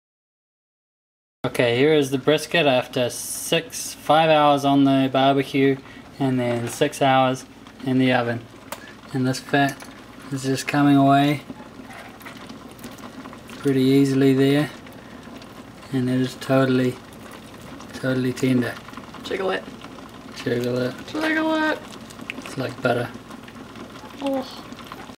Texas BBQ Brisket
6.5 lb angus brisket. Rubbed with a mixture of paprika, ground chili, salt, pepper and worcestershire sauce, and let sit in fridge overnight. 5 hours on the BBQ - with the hood down at ~210 degrees F, and the brisket on the cooler side. On the far right burner were hickory chips that were soaked overnight, then wrapped in aluminium foil with the ends open. It was then transferred to the oven,  some beer & more worcestershire sauce was poured over it, and it was covered in foil. Stayed in the oven for 6 hours at 250 F. Taken out and rested for 30 mins. If I did it again I'd put it on the smoker for the entire time. MORE SMOKE!